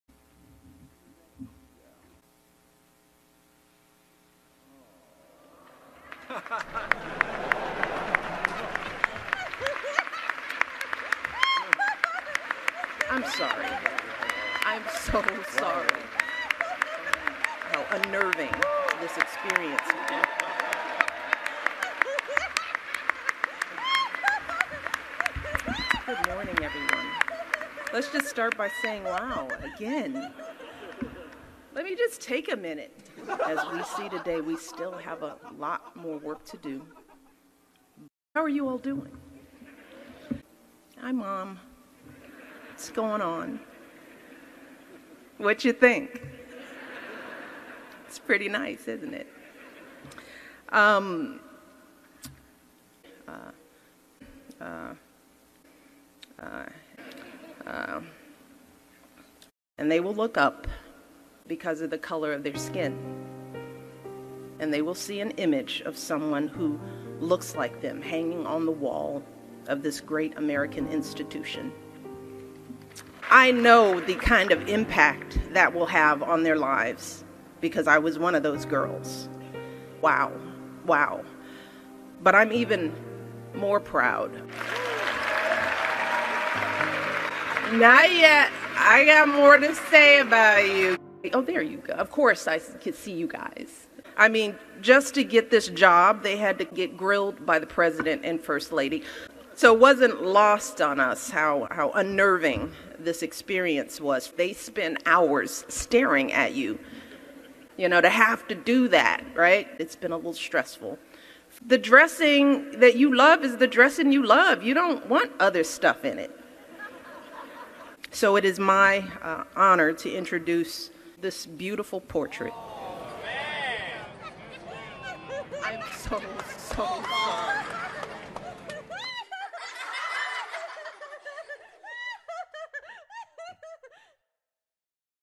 Official Portrait Painting of Michelle Obama Unveiled
Michelle Obama unveils her painted portrait. This video is as real as the Spacex launch videos.